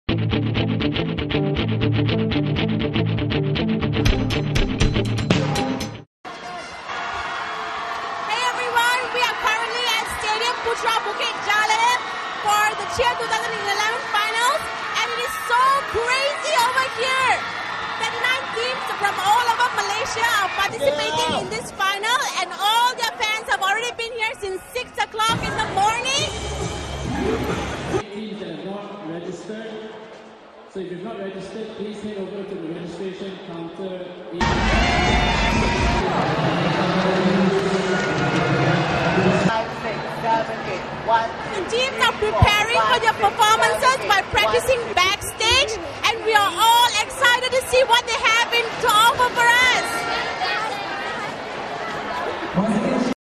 Cheer 2011: Behind the scenes
Switchup.tv goes behind the scenes of Cheer 2011. 39 teams from schools all over Malaysia are taking part in the finals.